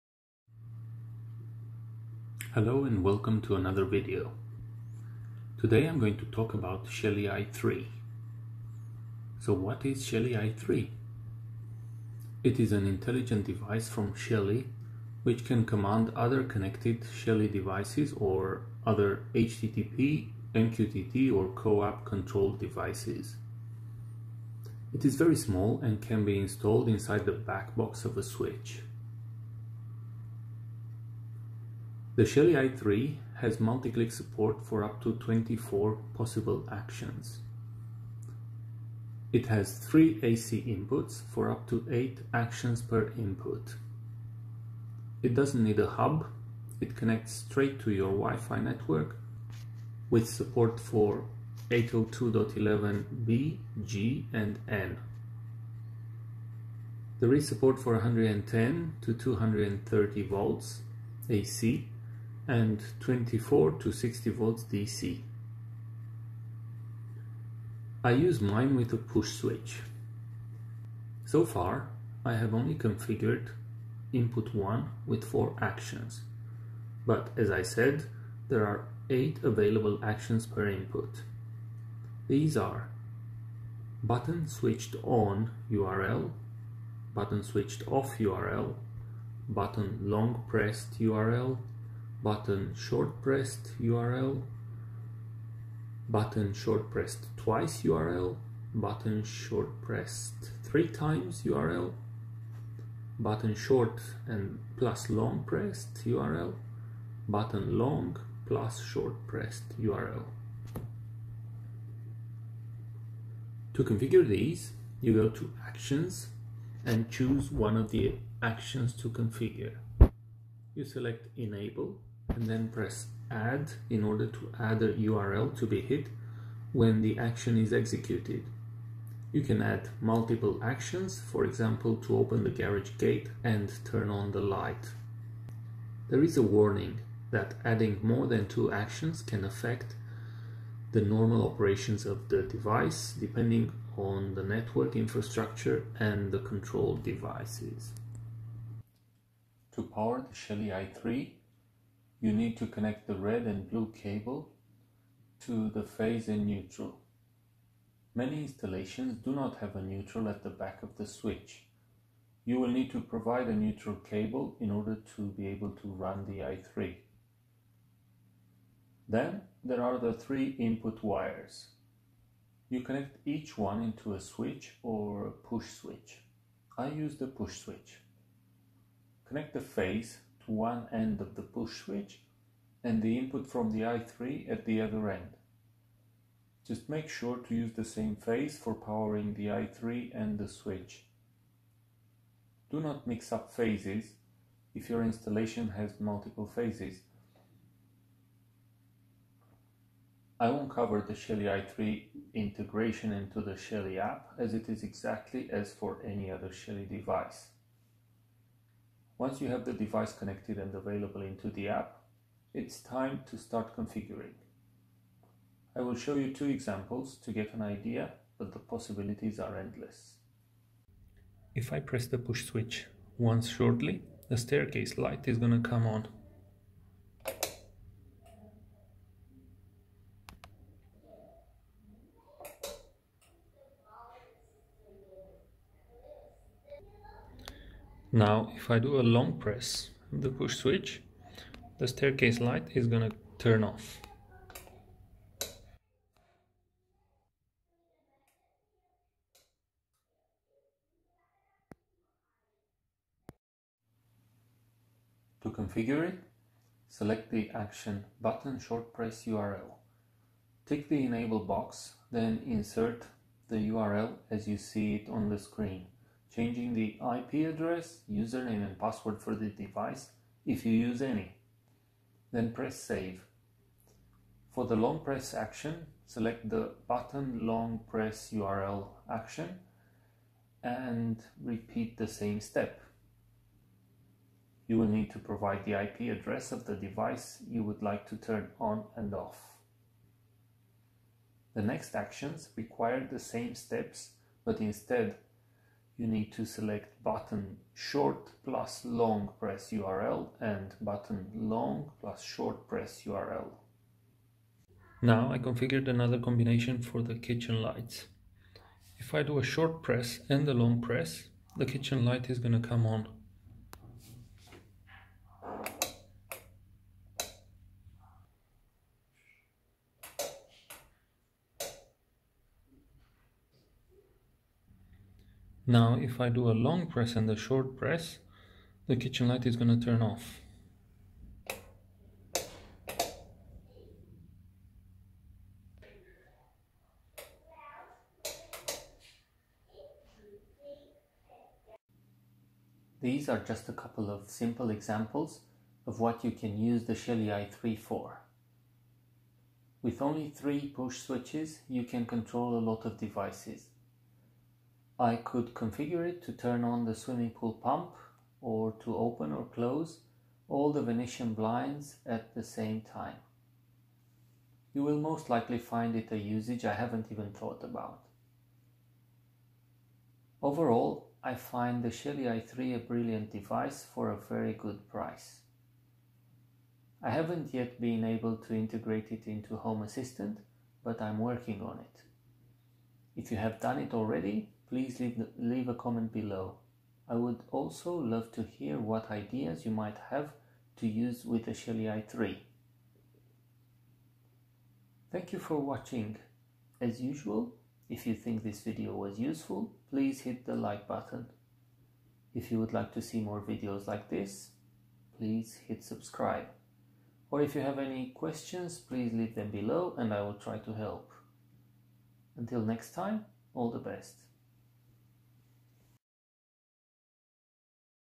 Shelly i3 - How to Connect and Configure the Shelly i3 smart device
In this video I show how to connect and configure a Shelly i3. The i3 is an intelligent device from Shelly which allows performing up to 24 actions from a maximum of 3 switches. It has 3 inputs, each input connects to a switch or push switch and it allows performing up to 8 actions per switch, such as short press, long press, double press, triple press or a combination of long and short presses. It connects to your network and the Internet using WiFi and supports WiFi b/g/n. Is has a very small form factor allowing hiding in a switch back box. There is one thing you need to keep in mind: it needs a Neutral in order to be able to function, so you will need to make sure you bring a neutral to the switch backbox where you intend to install it, if there isn't one already there.